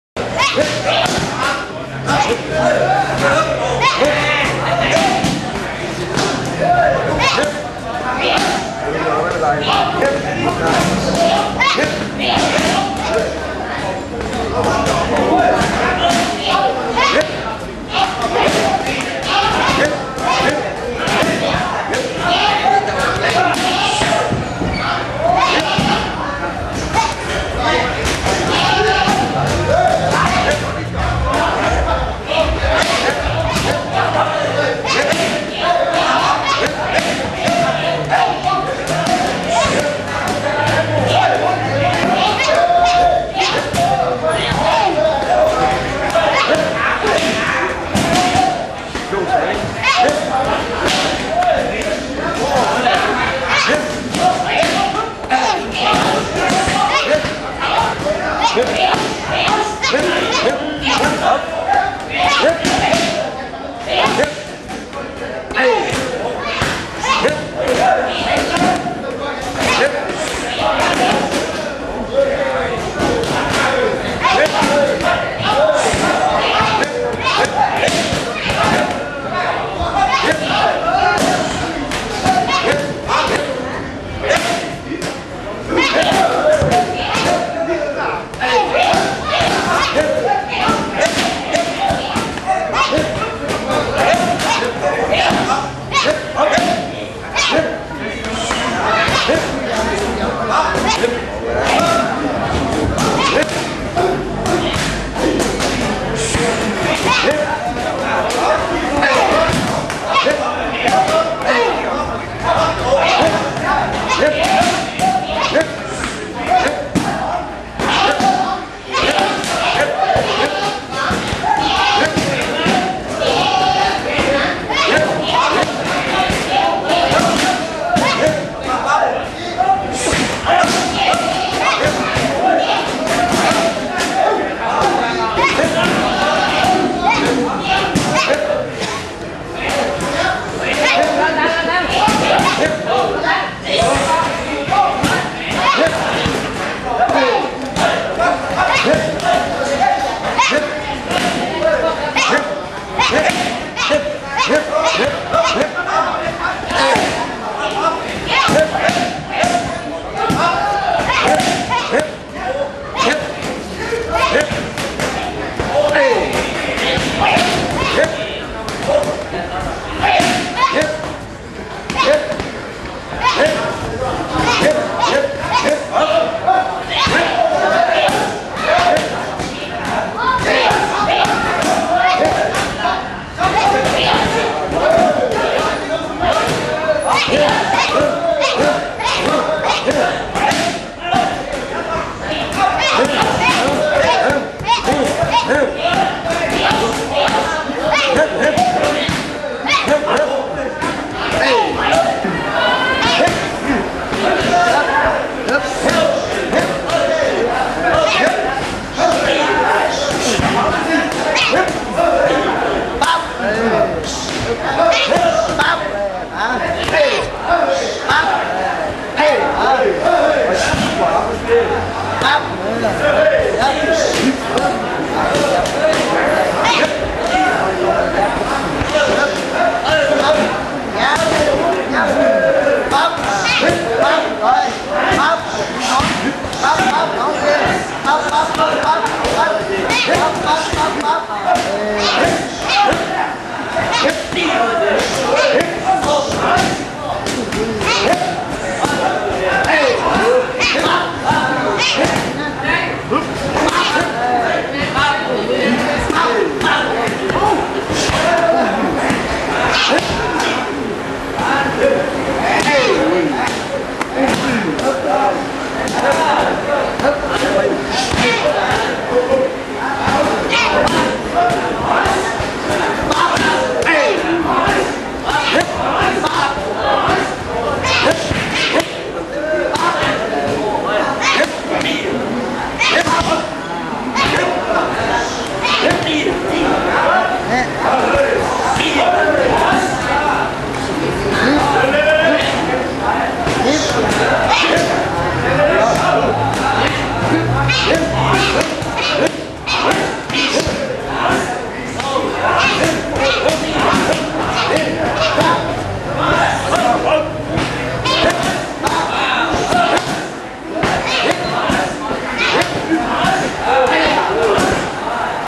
JangJao Fairtex *FAIRTEX BANGPLEE*
Jakhid Fairtex training his son, JangJao. The future of Muay Thai!
FAIRTEX BANGPLEE